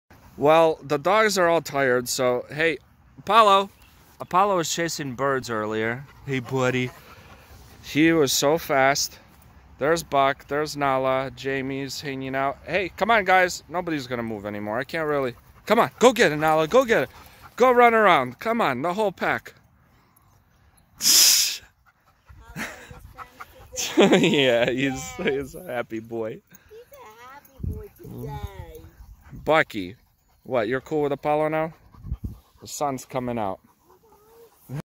Cane Corso and Pitbull meet Rottweiler
Cane Corso, Pitbull, and Rottweiler meet for first time.